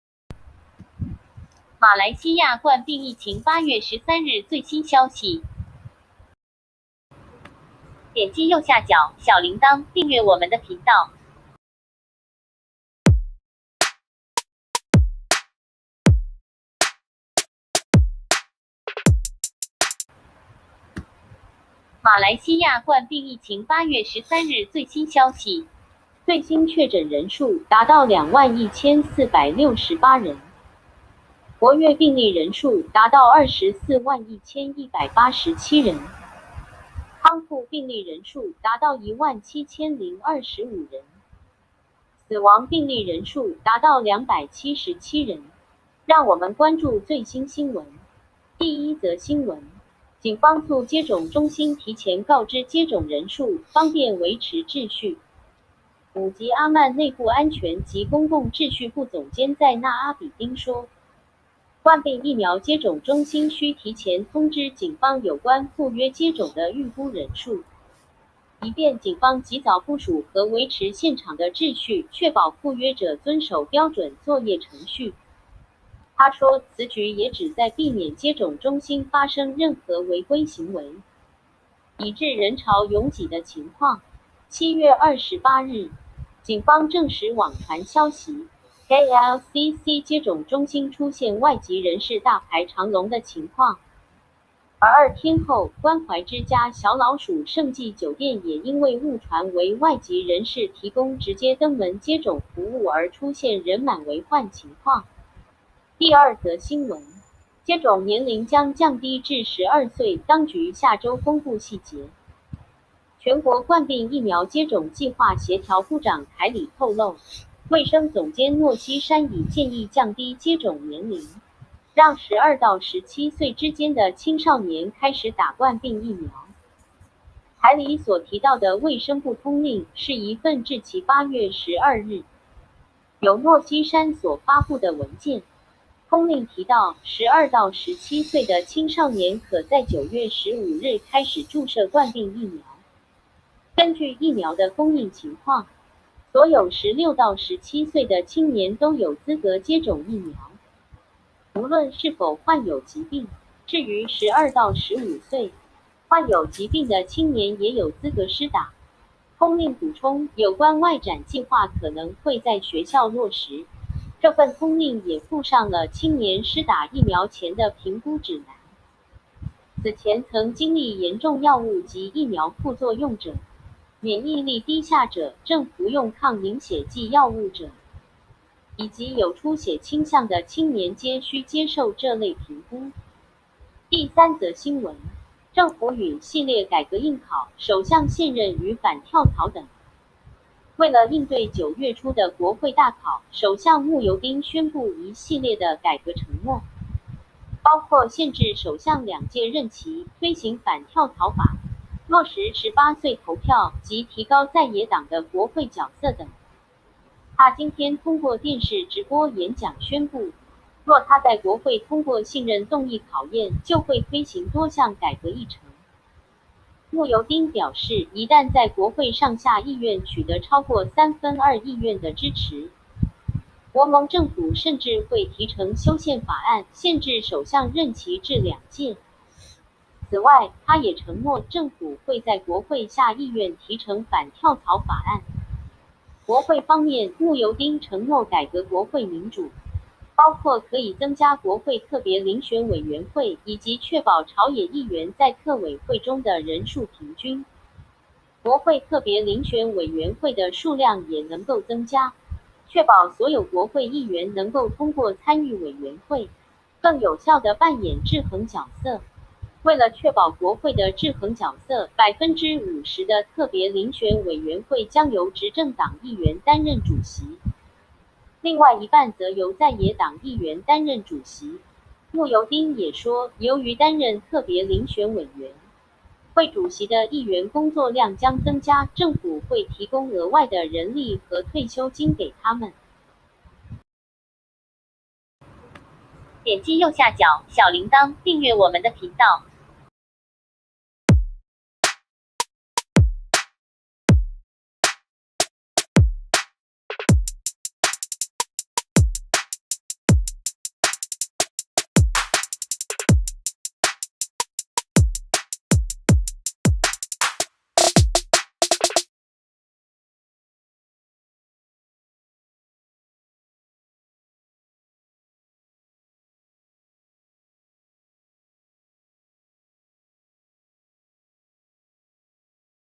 8 月 13 日
马来西亚冠病疫情 8 月 13 日最新消息
警方促接种中心提前告知接种人数，方便维持秩序
武吉阿曼内部安全及公共秩序部总监再纳阿比丁说，
冠病疫苗接种中心需提前通知警方有关赴约接种的预估人数，
以便警方及早部署和维持现场的秩序，确保赴约者遵守标准作业程序。
他说，此举也旨在避免接种中心发生任何违规行为，
以致人潮拥挤的情况。
7月28日，
警方证实网传消息，
KLCC接种中心出现外籍人士大排长龙的情况
而2天后，关怀之家@盛季酒店也因为误传为外籍人士提供直接登门接种服务而出现人满为患情况。
第二则新闻
接种年龄将降低至12岁，当局下周公布细节
全国冠病疫苗接种计划协调部长凯里透露，
卫生总监诺希山已建议降低接种年龄，
让12到17岁之间的青少年开始打冠病疫苗。
凯里所提到的卫生部通令，是一份志期8月12日，
由诺希山所发布的文件
通令提到，12到17岁的青少年可在9月15日开始注射冠病疫苗。
根据疫苗的供应情况，
所有16到17岁的青年都有资格接种疫苗，
无论是否患有疾病。
至于12到15岁，
患有疾病的青年也有资格施打。
通令补充，有关外展计划可能会在学校落实。
这份通令也附上了青年施打疫苗前的评估指南。
此前曾经历严重药物及疫苗副作用者、
免疫力低下者、正服用抗凝血剂药物者，
以及有出血倾向的青年皆须接受这类评估。
第三则新闻
政府允系列改革 应考 ：首相限任与反跳槽等
为了应对9月初的国会 大考 ，首相慕尤丁宣布一系列的改革承诺，
包括限制首相两届任期、推行反跳槽法、
落实18岁投票，及提高在野党的国会角色等。
他今天通过电视直播演讲宣布，
若他在国会通过信任动议考验，就会推行多项改革议程。
慕尤丁表示，一旦在国会上下议院取得超过三分二议院的支持，
国盟政府甚至会提呈修宪法案，限制首相任期至两届。
此外，他也承诺，政府会在国会下议院提呈反跳槽法案。
国会方面，慕尤丁承诺改革国会民主，
包括可以增加国会特别遴选委员会，以及确保朝野议员在特委会中的人数平均。
国会特别遴选委员会的数量也能够增加，
确保所有国会议员能够通过参与委员会，
更有效地扮演制衡角色。
为了确保国会的制衡角色，50%的特别遴选委员会将由执政党议员担任主席，
另外一半则由在野党议员担任主席。
慕尤丁也说，由于担任特别遴选委员
会主席的议员工作量将增加，政府会提供额外的人力和退休金给他们。